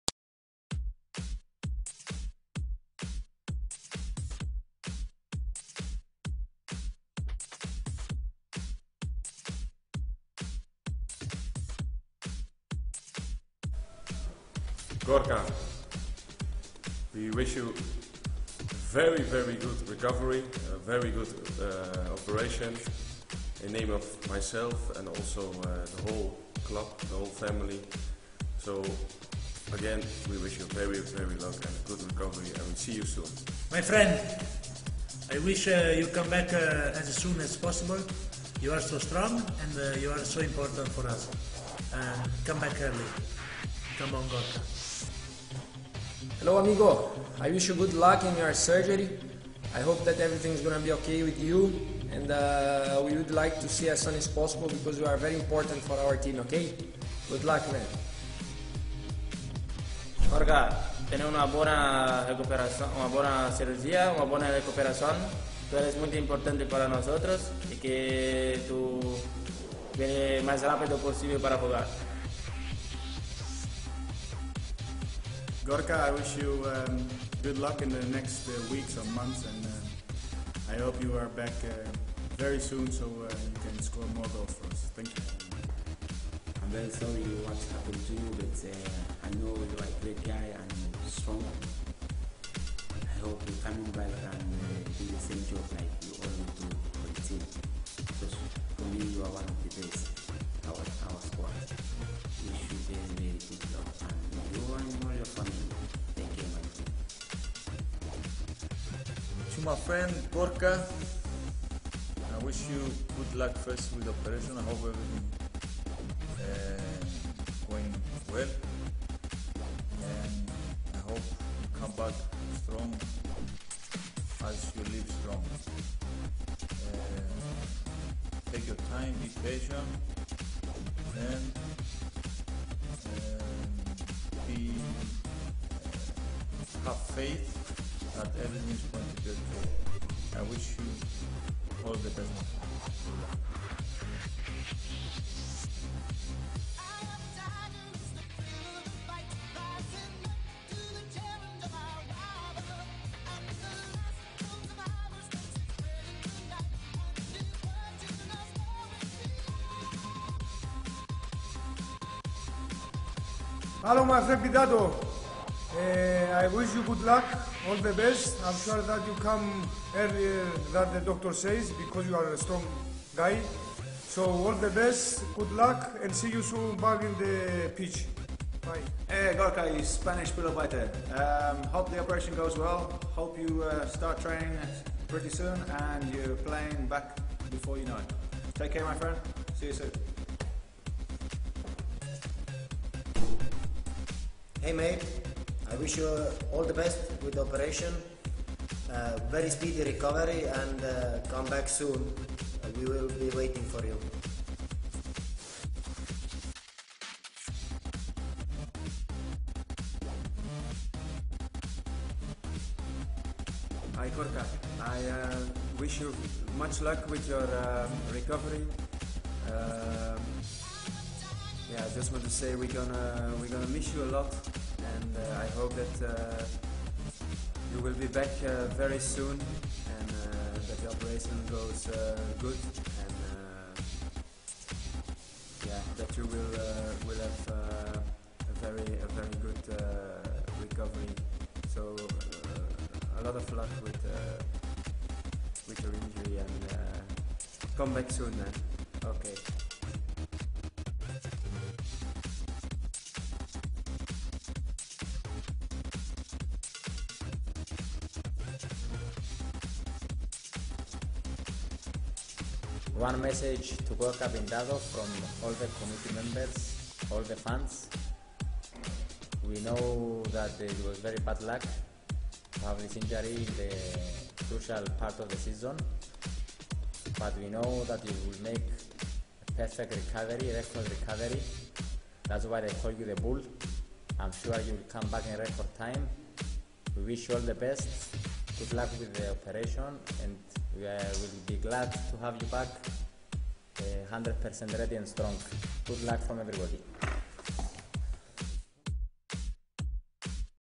Gorka Pintado Get well Soon
AEK Team and coaches , wish Gorka Pintado a good luck and speedy recovery for his knee operation.